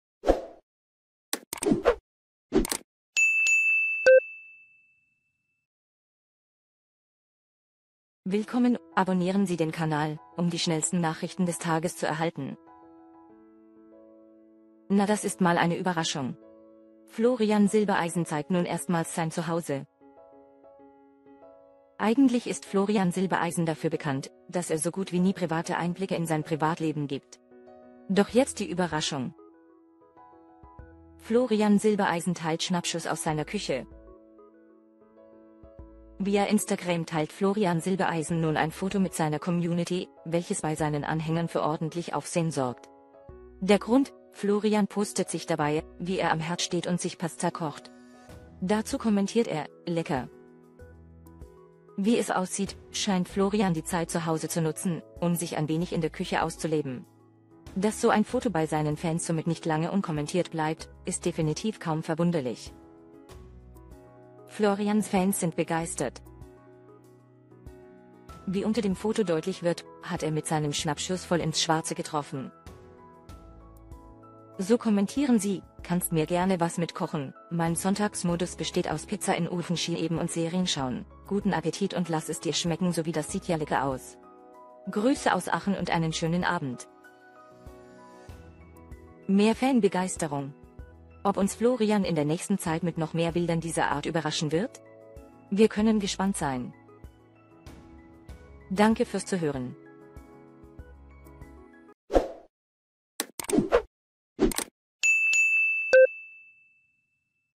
Florian Silbereisen Jetzt zeigt er erstmals sein zu Hause! Fans sind begeistert!
Na das ist mal eine Überraschung! Florian Silbereisen zeigt nun erstmals sein zu Hause...
✅Quelle : newstwgerman.de
*► Hinweis: TWGerman besitzt nicht alle in den Videos verwendeten Materialien.
Vielen Dank!
© Copyright by TWHK media - Do not Reup!